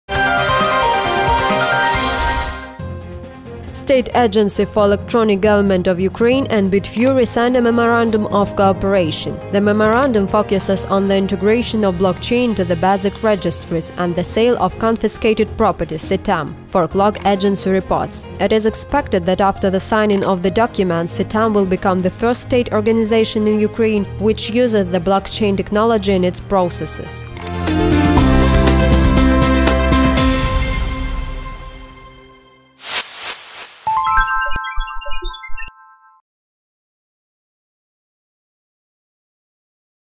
KCN EGov Agency and Ministry of Justice of Ukraine signed an agreement with BitFury
State Agency for Electronic Government of Ukraine and Bitfury signed a memorandum of cooperation. The memorandum focuses on the integration of bloсkchain into the basic registers and the sale of confiscated property "SETAM", Forklog agency reports. It is expected that after the signing of the document, "SETAM" will become the first state organization in Ukraine which uses the  blockchain technology in its processes.